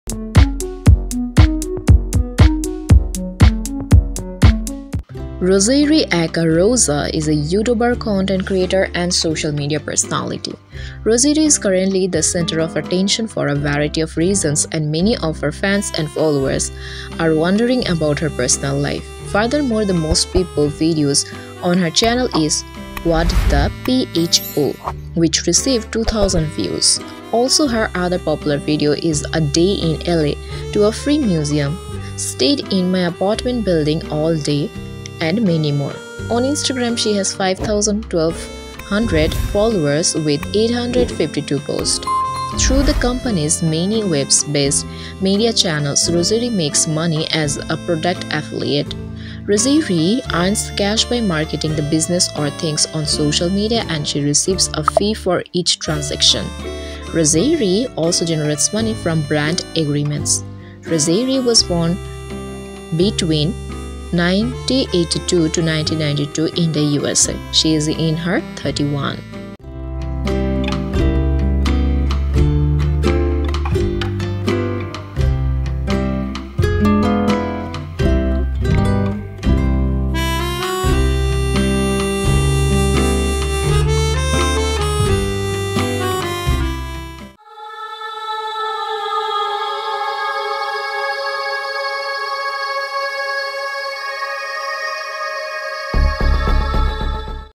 Roserayy Great Curvy Model Biography, Wiki, Plus Size Fashion, New Fashion Looks 2024
Roserayy aka Rosa is a Youtuber, Content Creator, and Social media personality. Roserayy is currently the center of attention for a variety of reasons, and many of her fans and followers are wondering about her personal life.

Our Channel